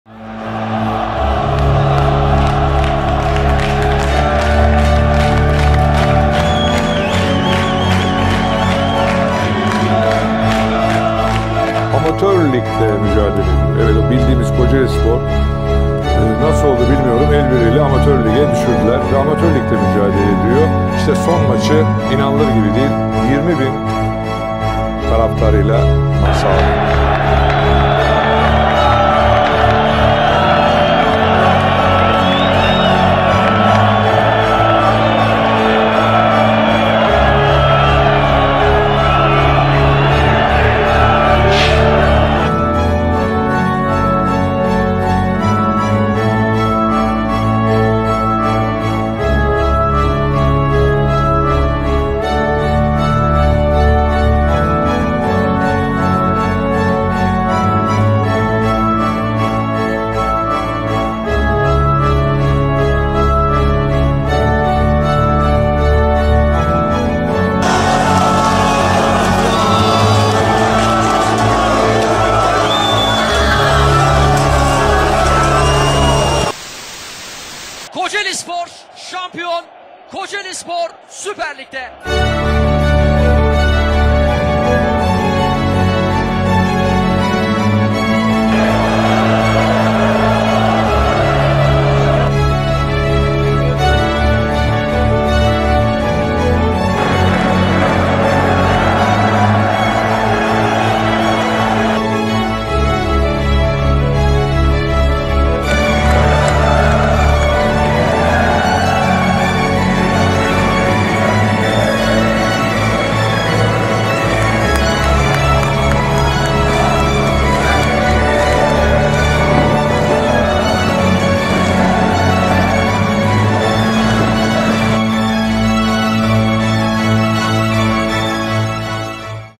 Çekilen onca acının bir anlamı var!
Çekilen onca acının bir anlamı var. Özlediğimiz güzel günler için şimdi sımsıkı sarılma zamanı!

Küllerimizden doğarak Süper Lig’e yürüdüğümüz bu günlere tanıklık edemeyen ve hayatta olmayan taraftarlarımız için son nefesimize kadar mücadele edeceğiz!